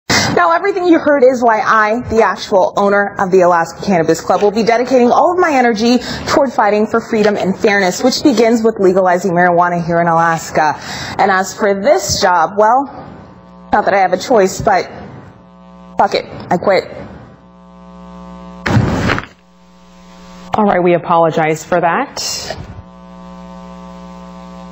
"Fuck It, I Quit" - News Reporter Quits on Live TV
You don't see this every day.  How she quits is what really makes it newsworthy.

To be fair though, how many big time names have come out of Alaska local television?  Sarah Palin comes to mind and the less said about her, the better.

No challenge to ownership is implied.